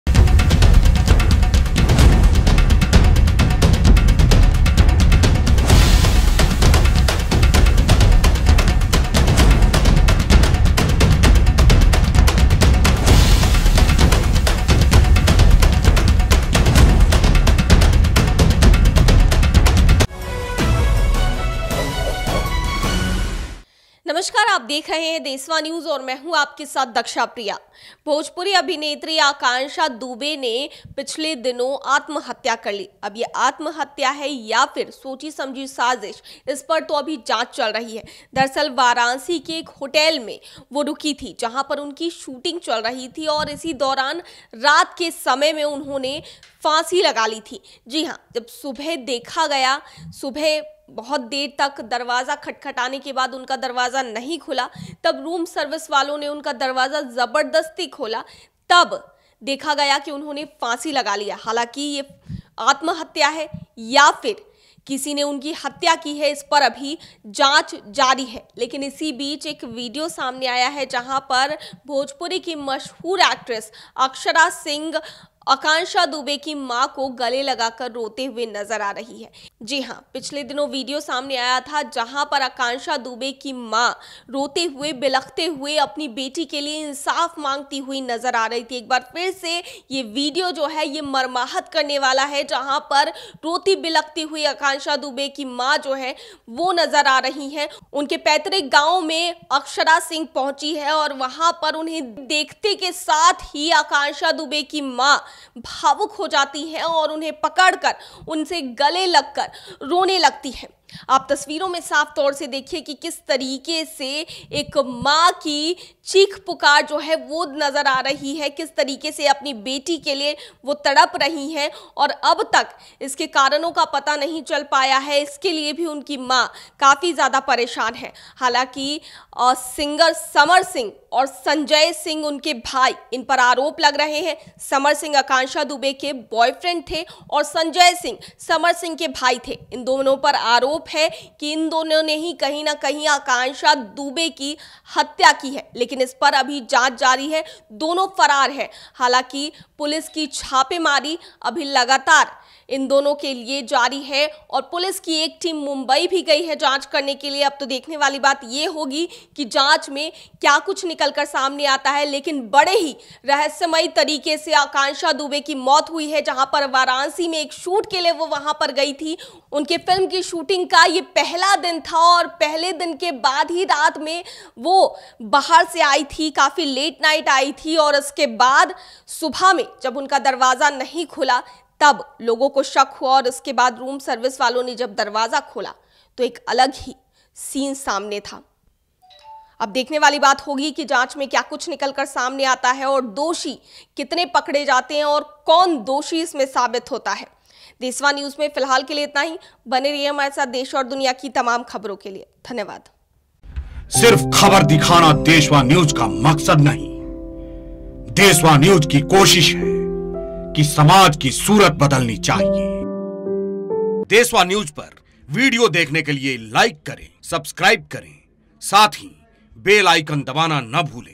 अभी अभी AKSHARA पहुंच गयी AKANKSHA DUBEY के घर, माँ से लिपट कर खायी कसम, नहीं छोडूंगी आरोपी SAMAR को
अभी अभी AKSHARA SINGH पहुंच गयी AKANKSHA DUBEY के घर, माँ से लिपट कर खायी कसम, नहीं छोडूंगी आरोपी SAMAR SINGH को